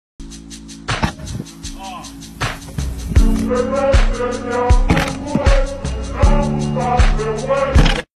guy puts knives around the backboard
Guy puts spikes and knives around basketball backboard